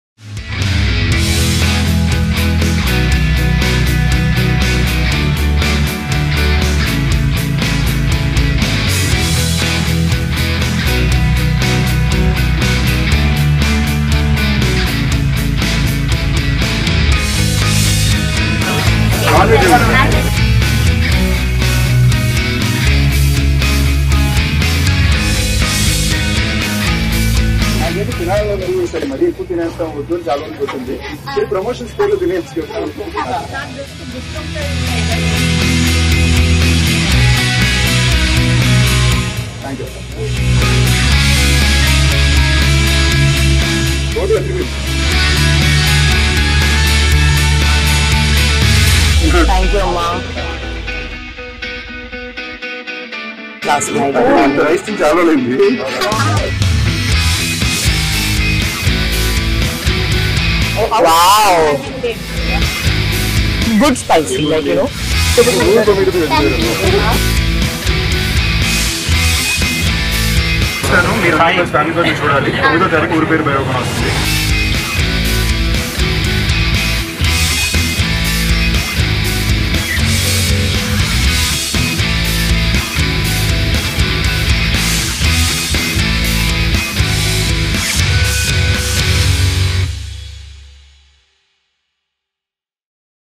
Kumari Aunty Serving Food to Ooru Peru BhairavaKona Movie Team | Sundeep Kishan and Varsha Bollamma
Watch :  Kumari Aunty Serving Food to Ooru Peru BhairavaKona Movie Team | Sundeep Kishan and Varsha Bollamma